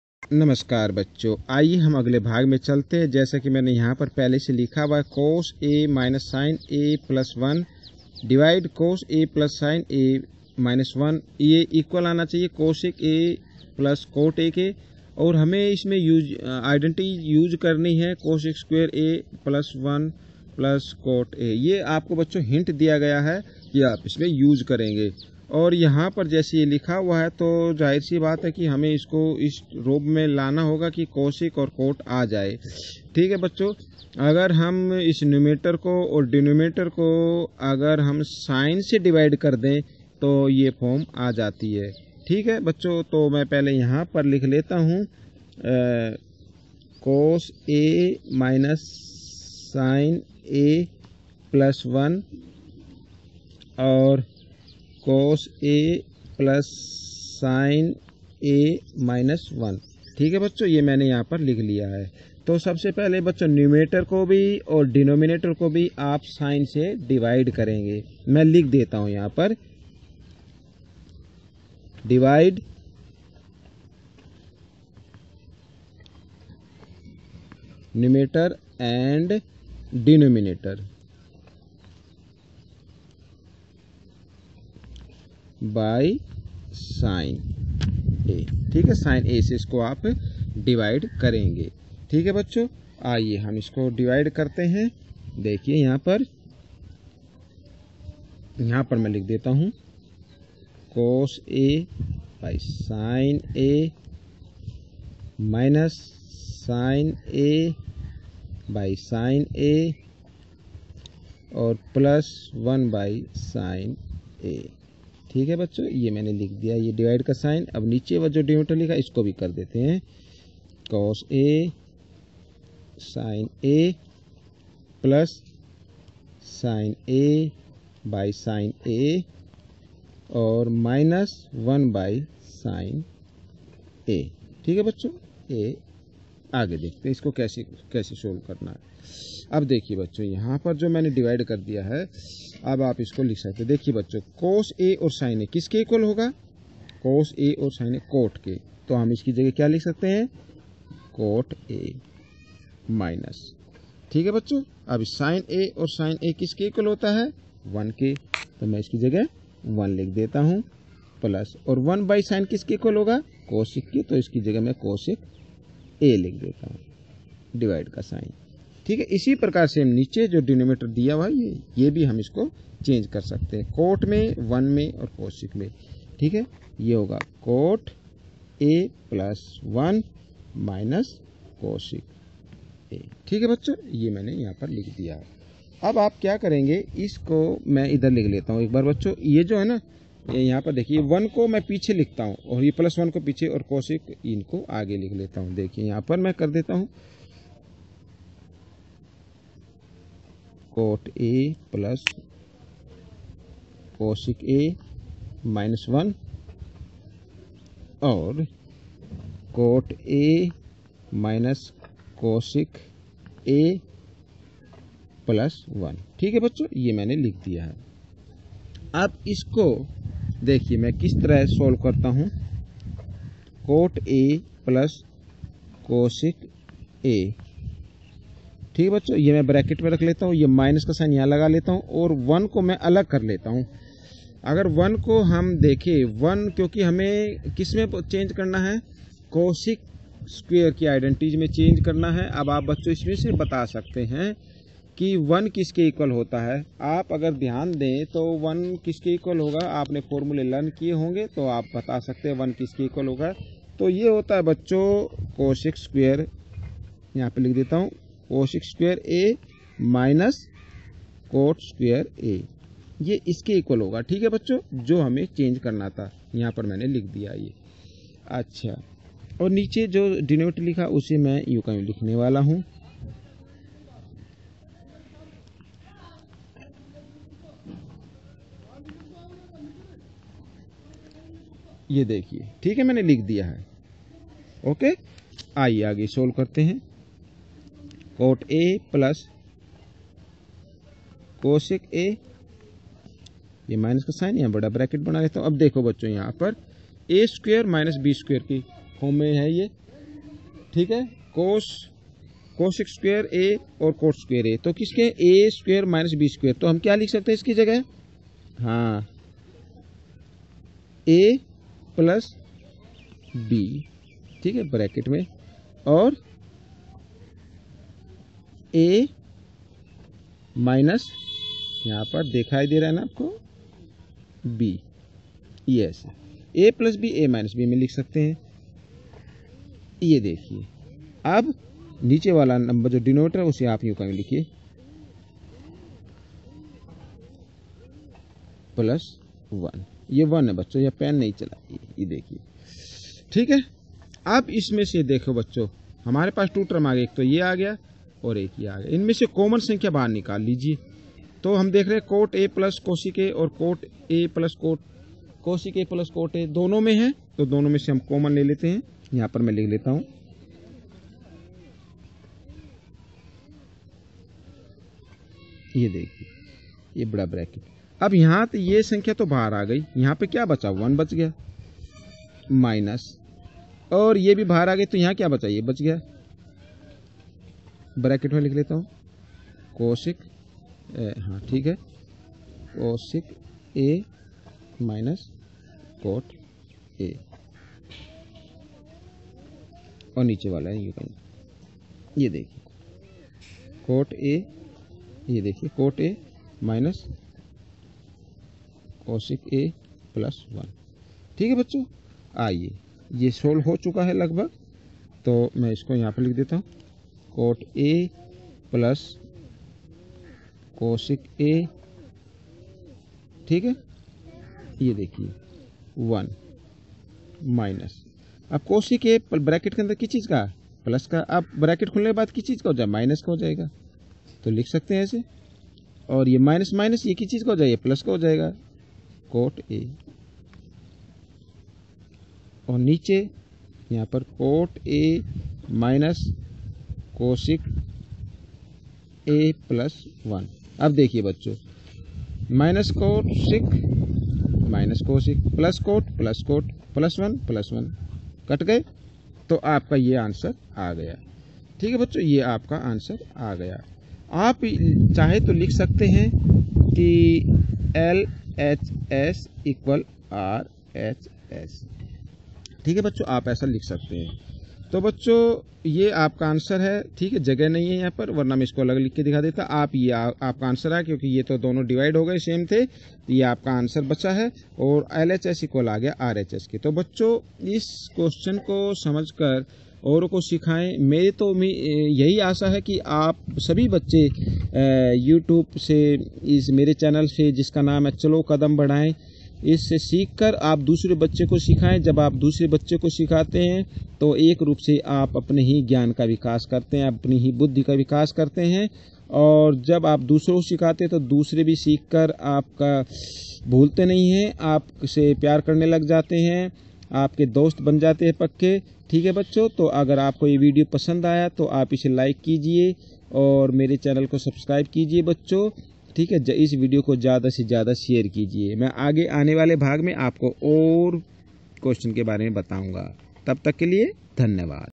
HOW TO SOLVE TRIGONOMETRY IDENTITIES CLASS 10TH PART # 8/20. hindi/english.
chalokadambadaye@gmail,com. how to solve trigonometry identities class 10th part # 8/20. this video for every students who read in class 10th.